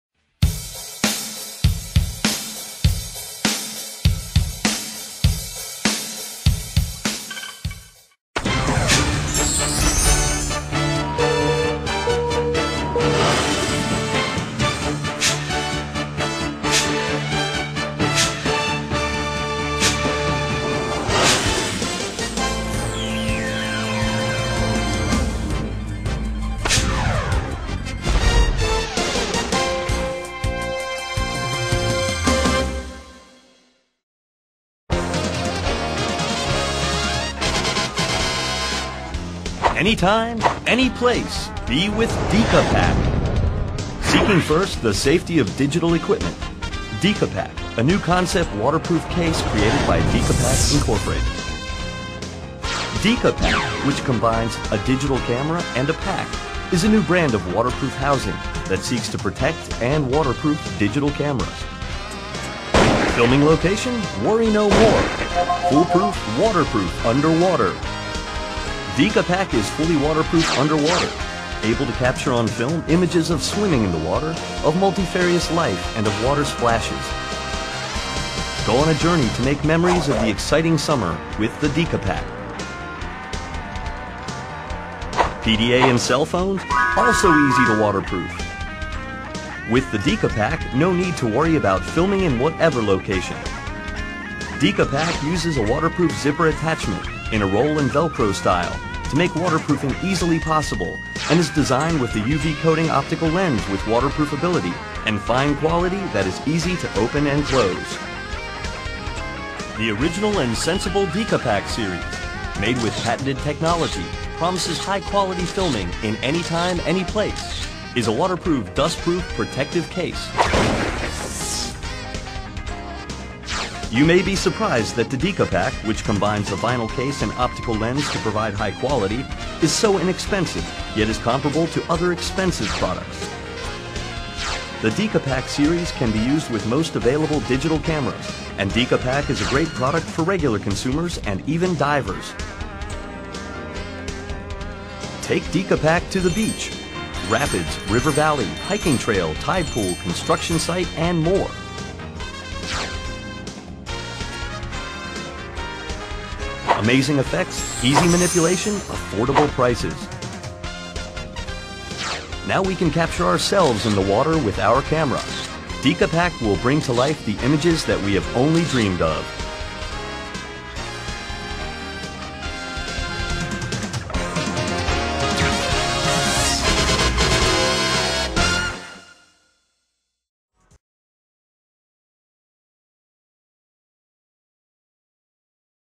Dicapac - Waterdichte camera case - ToyzAndGiftz
Een waterdichte beschermhoes voor je digitale fotocamera! Gegarandeerd waterdicht tot 5 meter.

De Dicapac buidels zijn gemaakt van vinyl en beschikken over een waterdichte sluiting. Het product biedt een uitstekende optische kwaliteit zowel onder als boven water door een optisch UV-lensje of venster.

De waterdichte sluiting is gegarandeerd d.m.v. dichtritsen, oprollen en met velcro dichtplakkers van de opening.

Tevens zit er een UV-coating over de lens voor een optimale fotokwaliteit. Ook het openen en sluiten van de lens is zeer eenvoudig en een uitzoomende lens is geen enkel probleem!

Uiteraard kan Dicapac ook gebruikt worden ter bescherming van de camera bij bijvoorbeeld een bezoek aan het strand of een waterpark en/of zwembad.

Alle knoppen van de camera kunnen makkelijk bediend worden door de flexibele eigenschappen van de Dicapac!